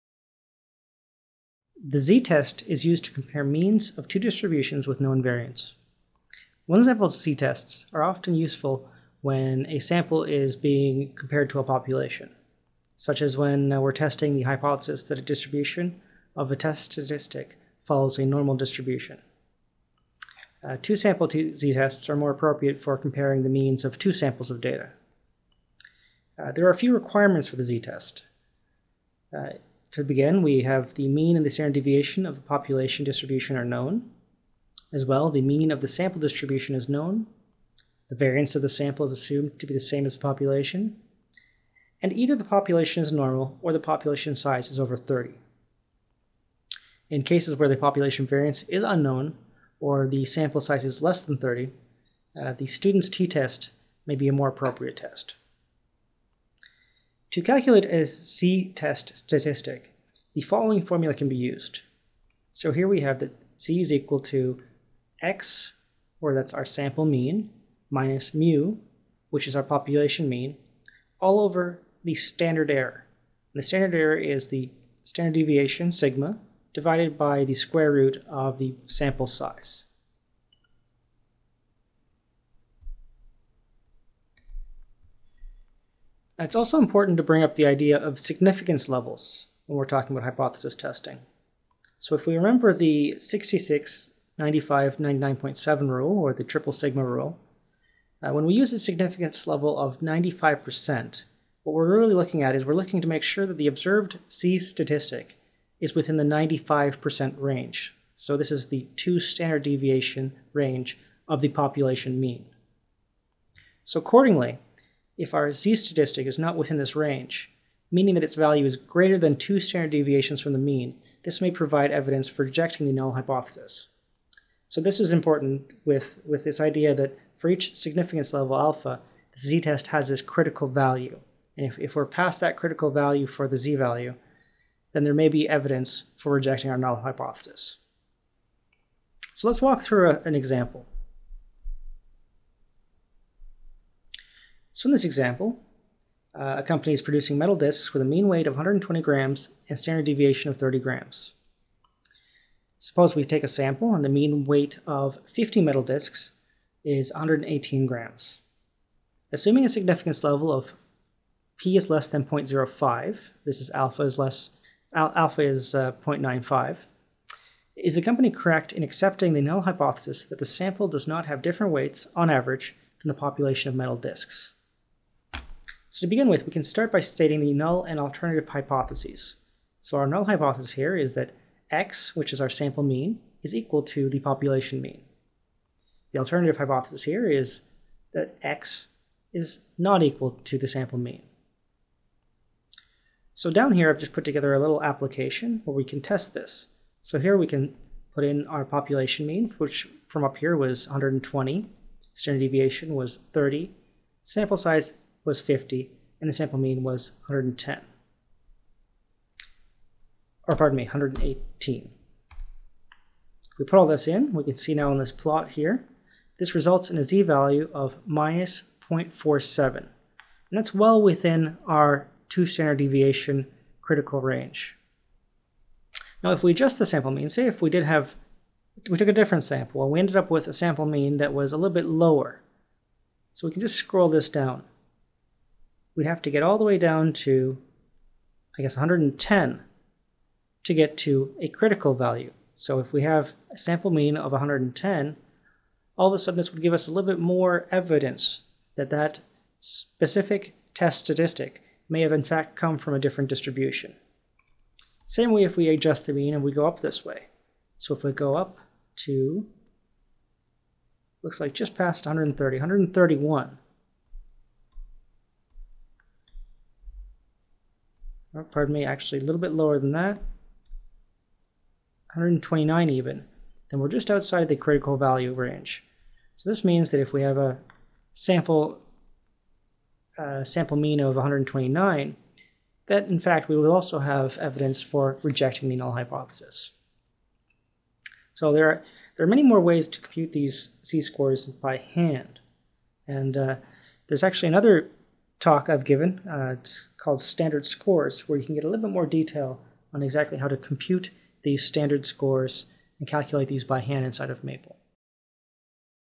Inferential Statistics with Maple: Z-tests
The z-test is used to compare means of two distributions with known variance. In this video, see an example of applying the one sample z-test to a sample of data. In addition, see interactive demonstrations on the boundaries for critical values for z-statistics.